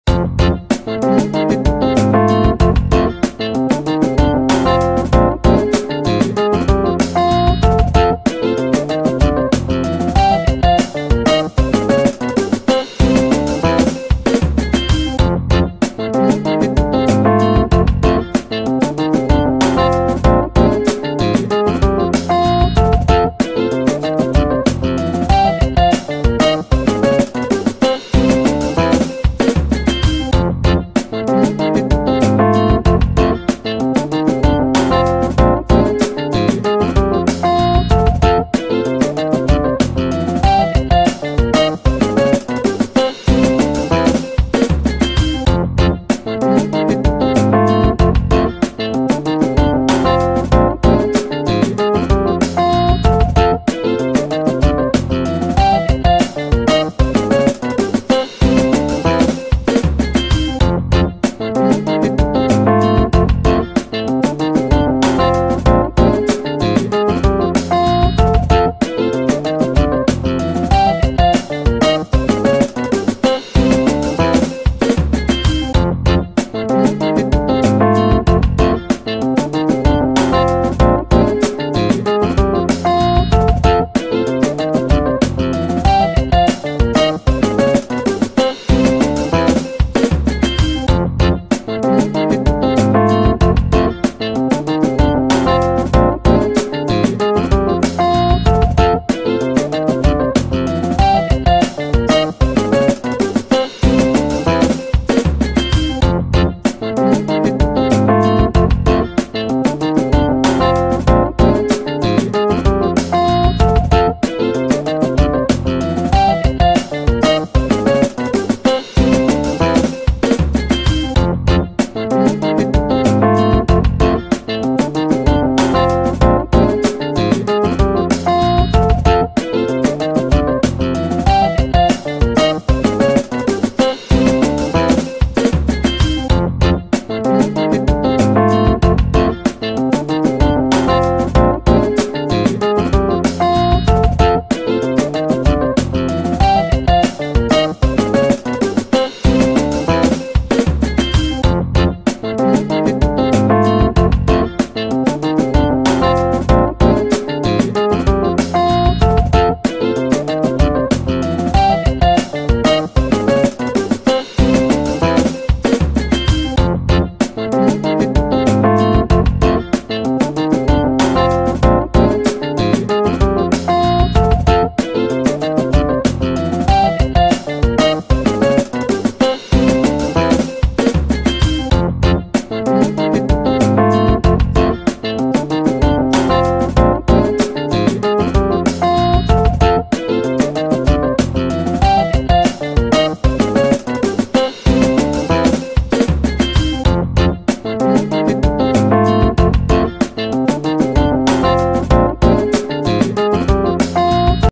Cara Mudah Menghilangkan IKLAN BBM android root tanpa aplikasi
Begini cara menghilangkan iklan di BBM tanpa aplikasi pada smartphone android rooted